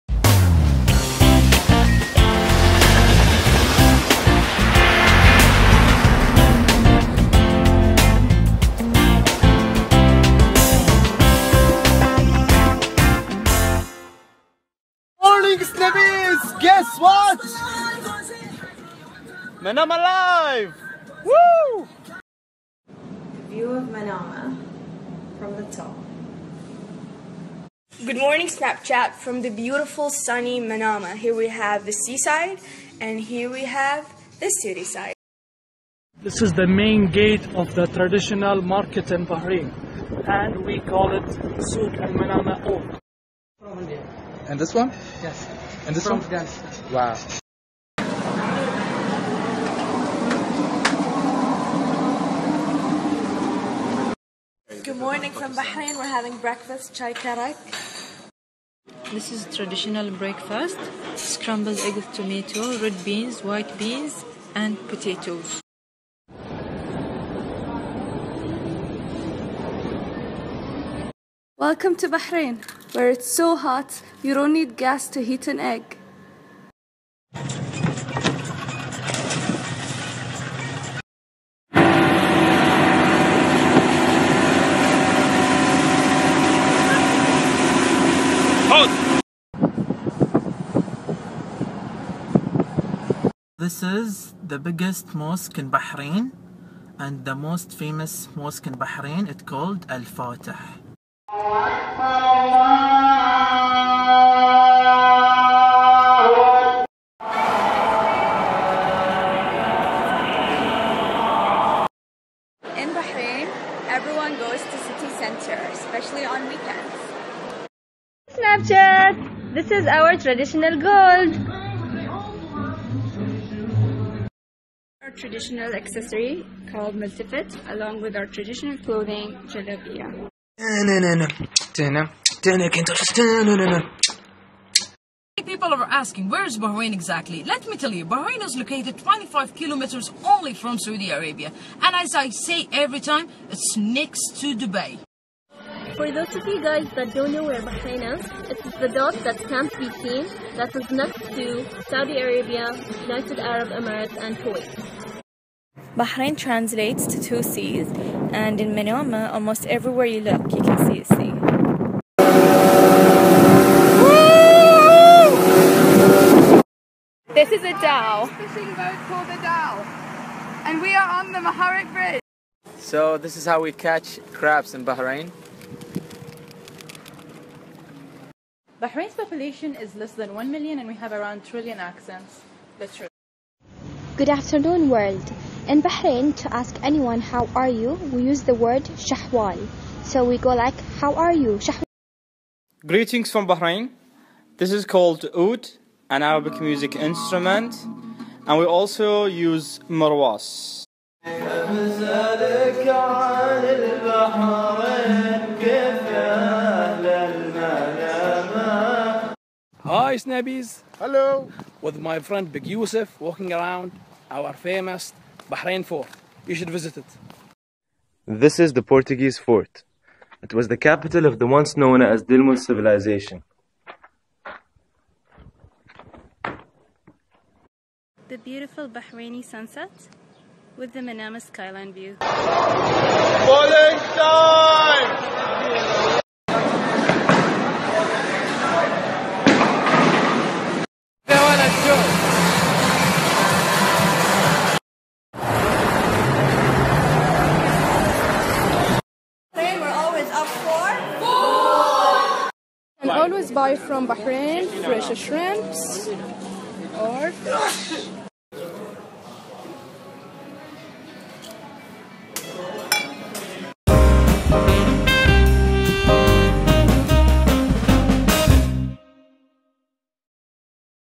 Day in the life of living in Manama Bahrain 2015
Watch how the people of Manama Bahrain live. Learn about the local tradition and the culture of those who live in the beautiful country of Bahrain.

If you are thinking of visiting Bahrain, it's a great video to watch to learn about the must see places.